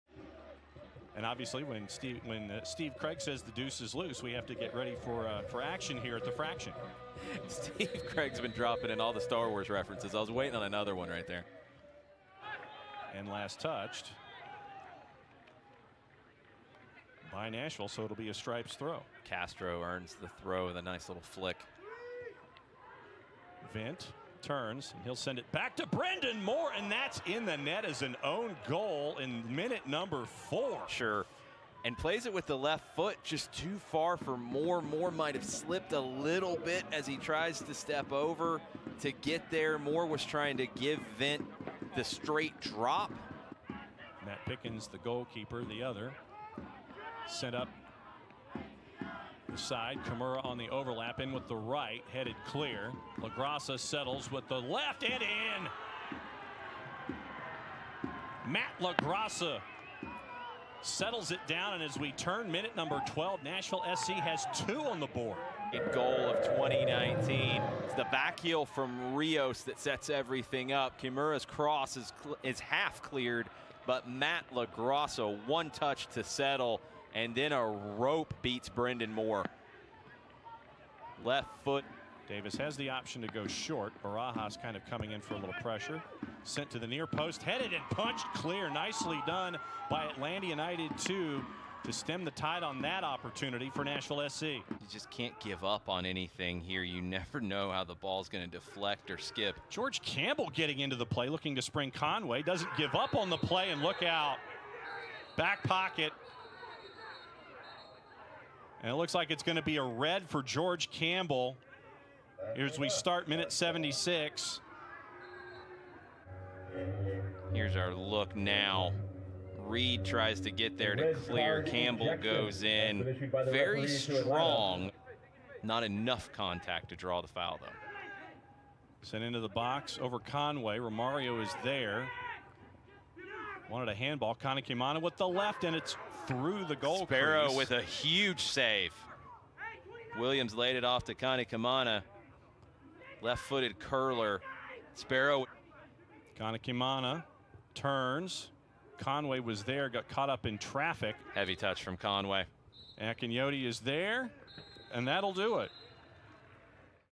HIGHLIGHTS: ATL UTD 2 vs Nashville SC | May 4, 2019
ATL UTD 2 falls short against Nashville SC on a rainy night in Kennesaw.